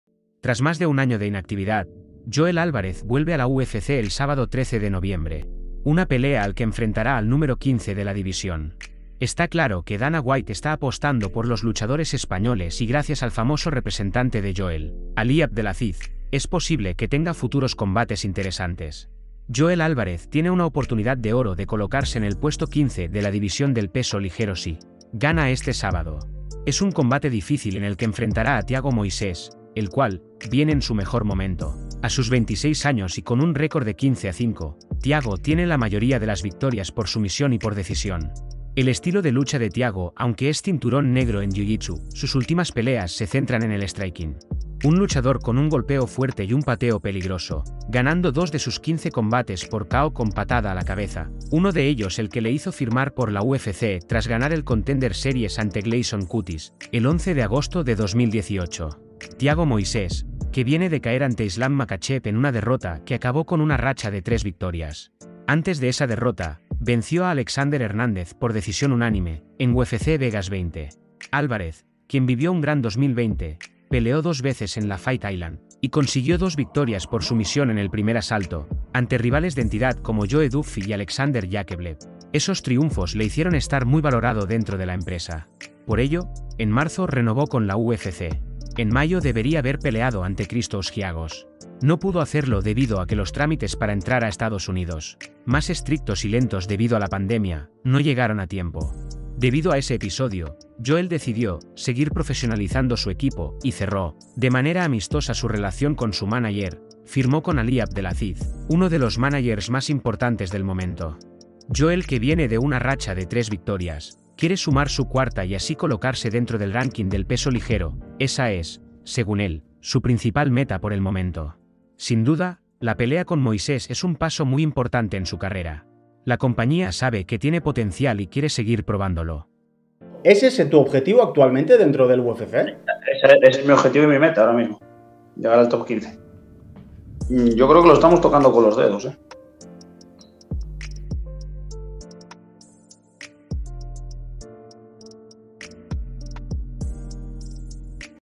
¡¡JOEL ÁLVAREZ A POR EL TOP 15!!
Joel Álvarez volverá a la jaula de la UFC tras más de un año sin pelear. El asturiano se enfrentará a Thiago Moisés, número 15 del ligero, el 13 de noviembre.